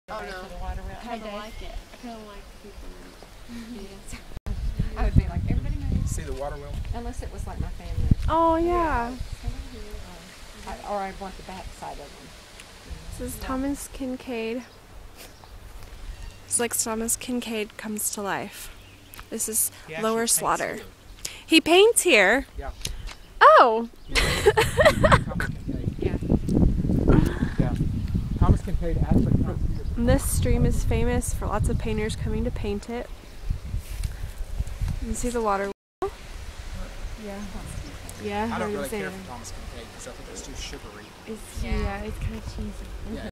The town that Thomas Kinkaide paints
Lower Slaughter in the Cotswolds, England